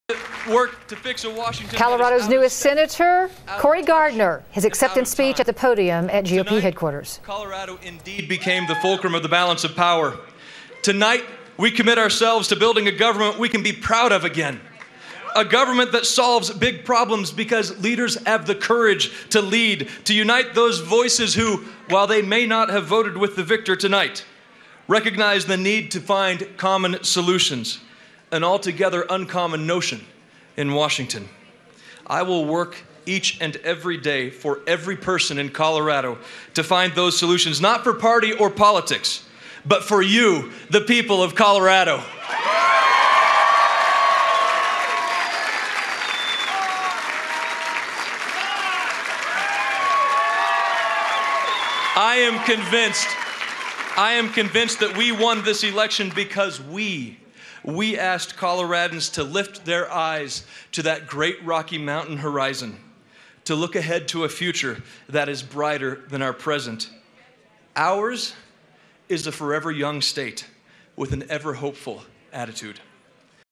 Gardner: I'll work every day for the people of Colorado
Republican Cory Gardner has defeated Democrat Mark Udall in a heated U.S. Senate race in Colorado.